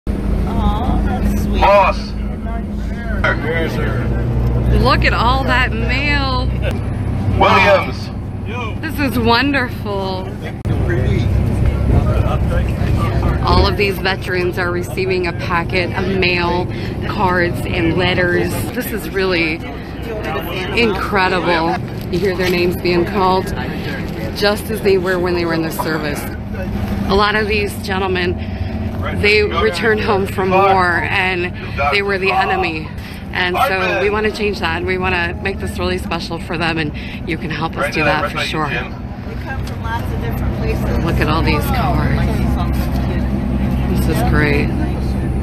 Twin Tiers Honor Flight Mail Call
Twin Tiers Honor Flight Mission 6 veterans receive mail call.